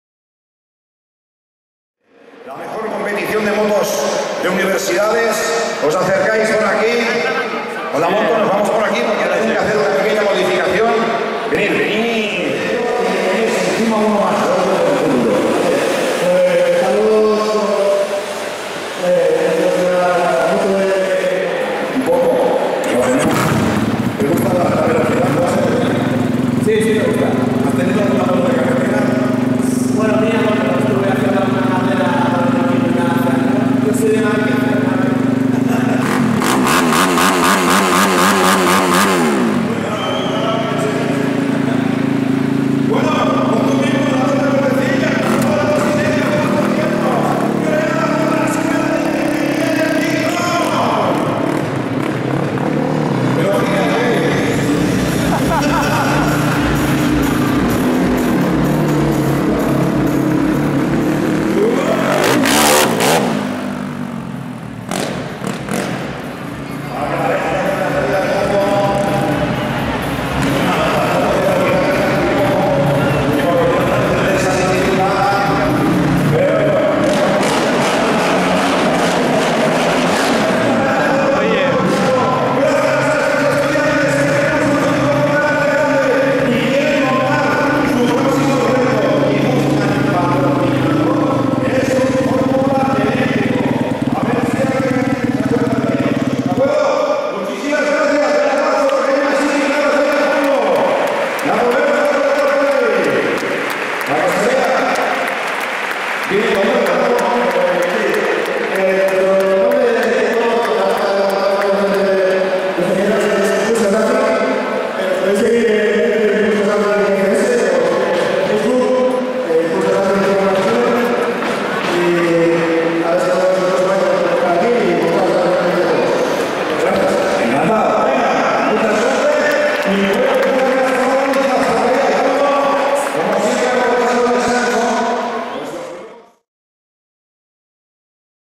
Nuestro equipo MS3 Uniovi en la Copa Burn
El Loco Miralles prueba nuestra moto en la presentación en la Copa Burn.
28 de diciembre de 2014,  Palacio de Deportes de Gijón.